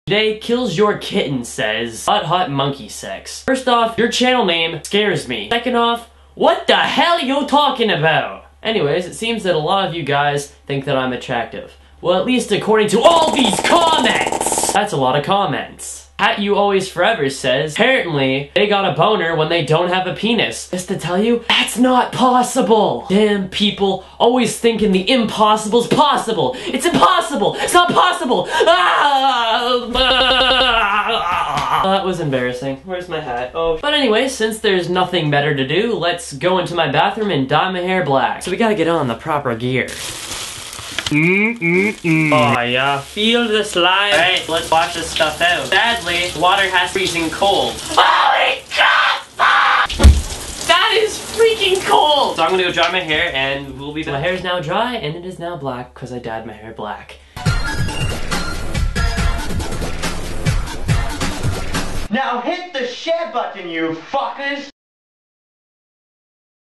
COLD Shower BALLS Dyeing My Hair Black (Hanging With Eric, EP #5)
cold shower balls allot of you guys think that i am attractive acording to all these comments  the imposeable is not posable since there is nothing better to do i go into my bathroom and dye my hair black